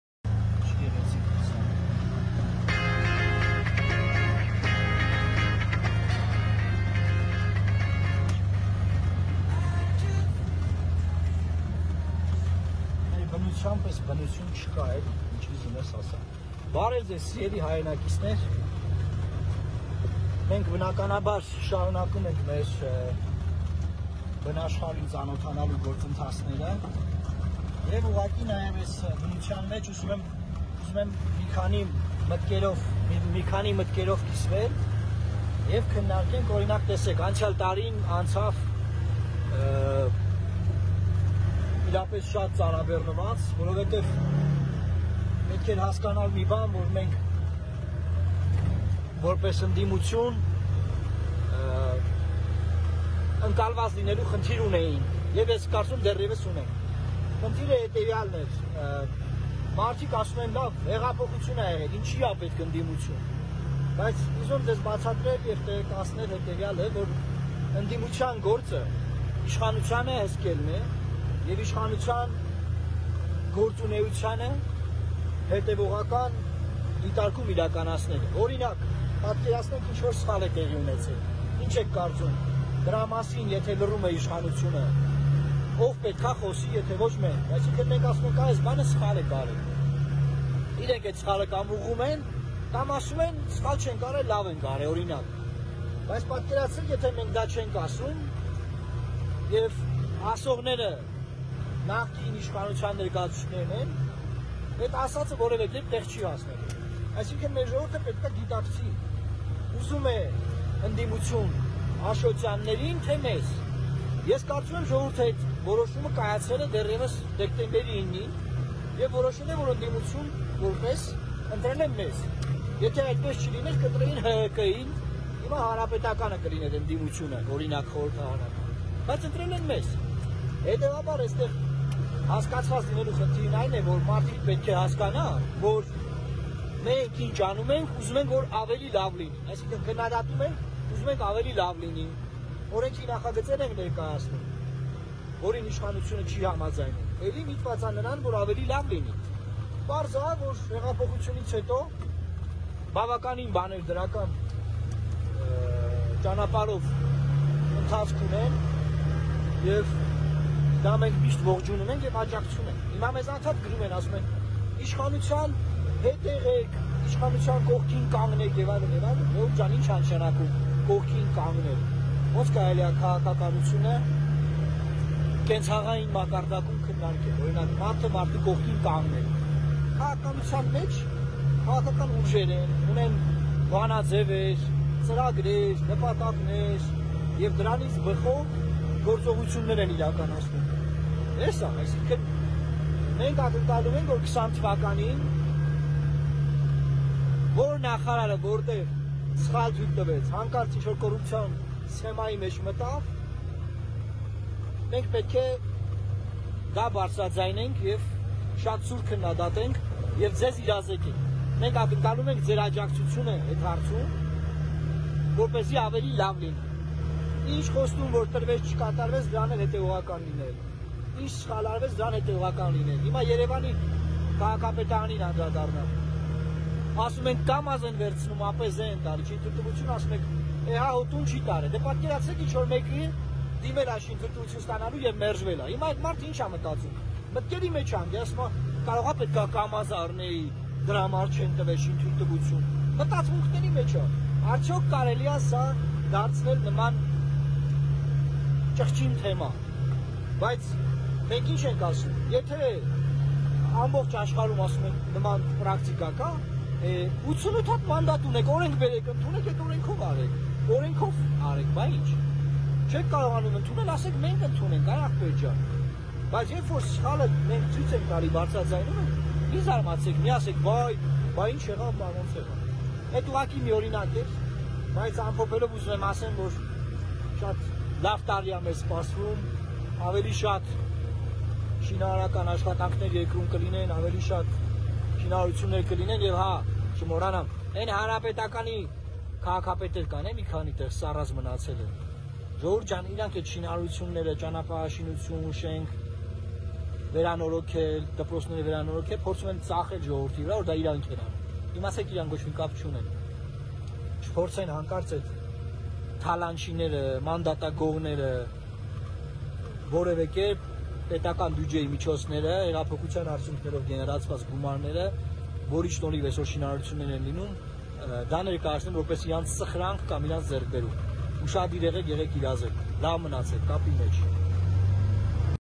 Խոհեր ընդդիմության դերի ու նշանակության մասին
«Լուսավոր Հայաստան» կուսակցության նախագահ էդմոն Մարուքյան։ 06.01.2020թ.